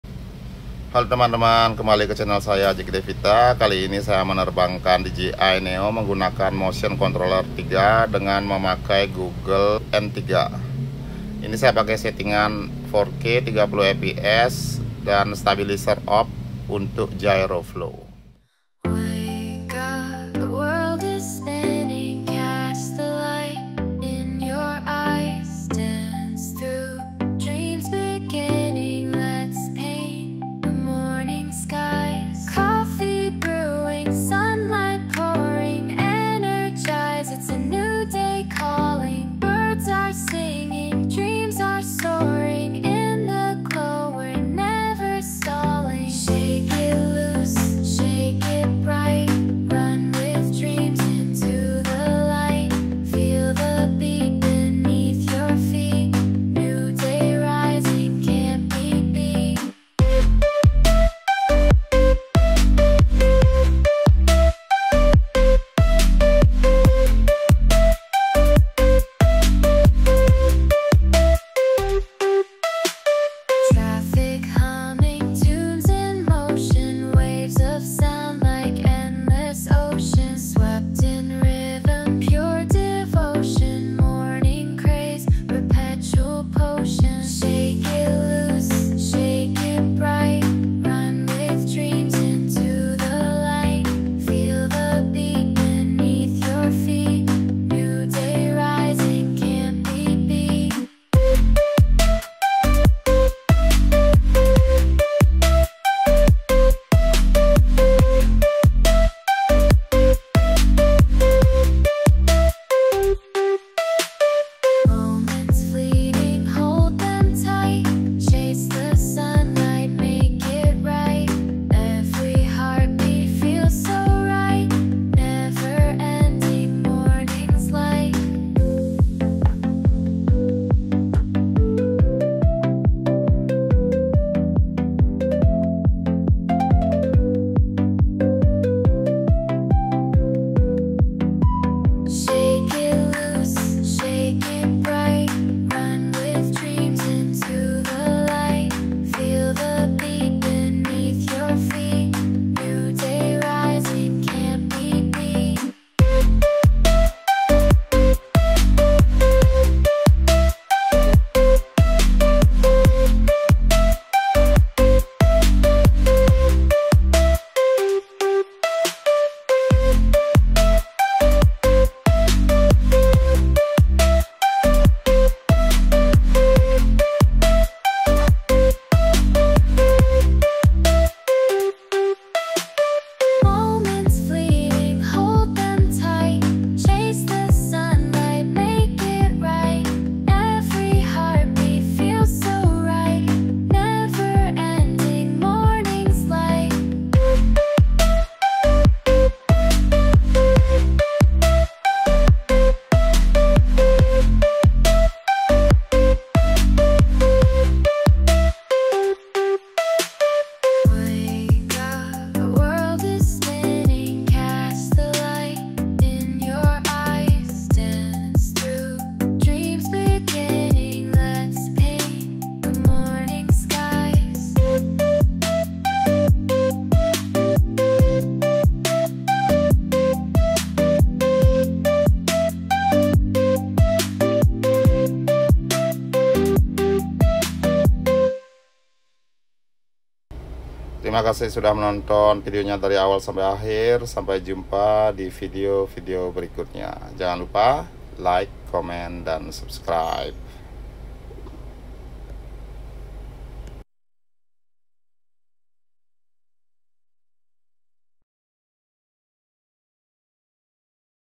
DJI Neo Sport Mode
Fly with DJI Motion 3 Controller and DJI Googles N3 ( head tracking )